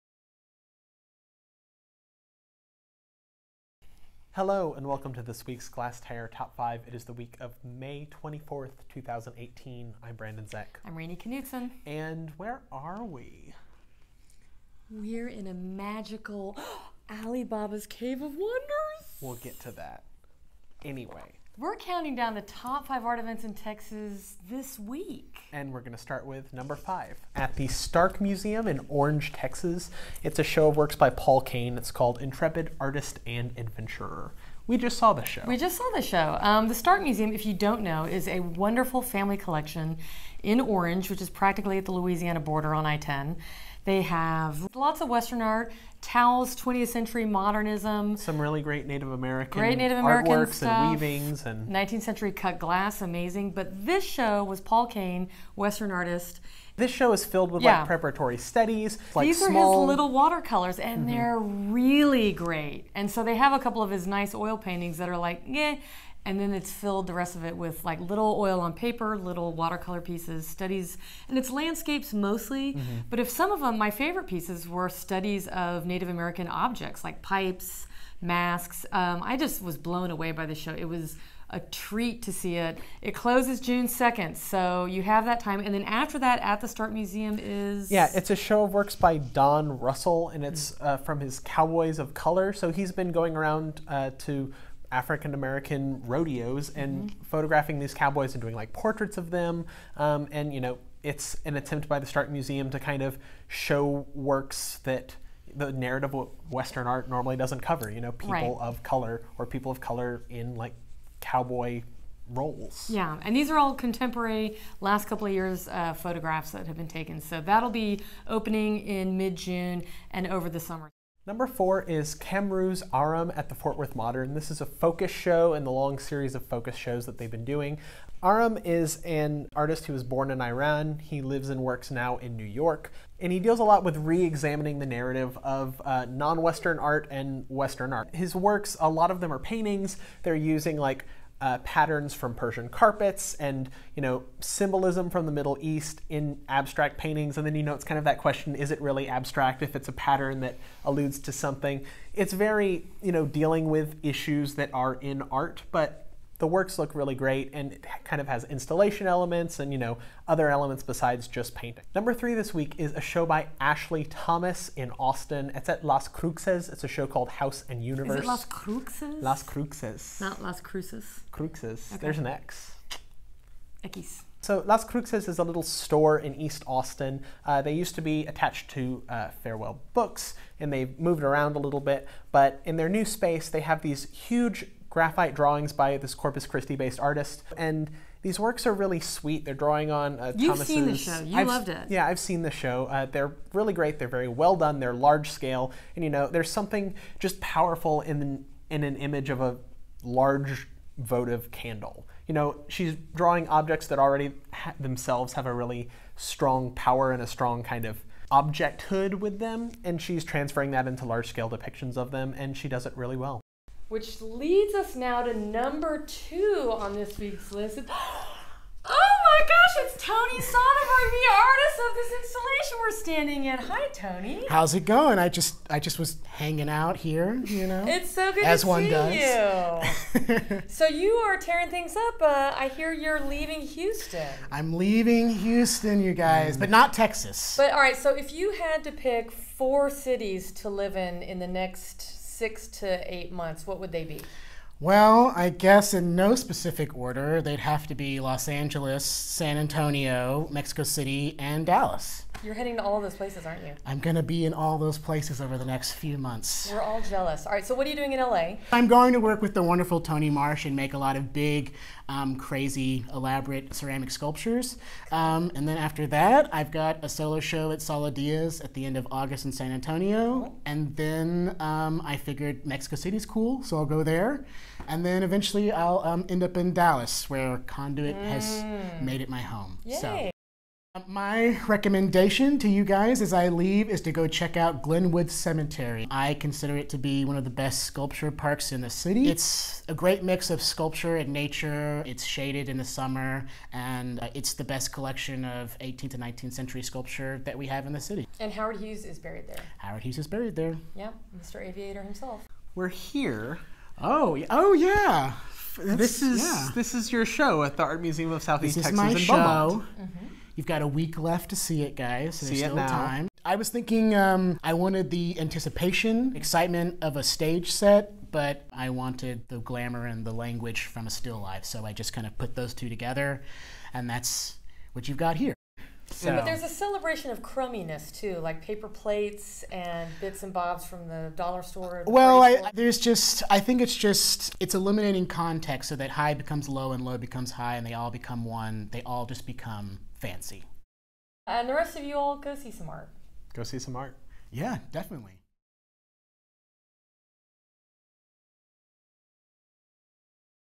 Glasstire Top Five: May 24, 2018 with Anthony Sonnenberg
Rainey Knudson, Brandon Zech, and special guest Anthony Sonnenberg on his extravagant installation in Beaumont, cowboy art in Orange, and an unexpected sculpture park in one of Houston’s most prominent cemeteries.

5. Paul Kane @ Stark Museum of Art, Orange
4. FOCUS: Kamrooz Aram @ Modern Art Museum of Fort Worth
3. Ashley Thomas: House and Universe @ Las Cruxes, Austin
2. Glenwood Cemetery in Houston
1. Anthony Sonnenberg: Still Stage, Set Life @ Art Museum of Southeast Texas, Beaumont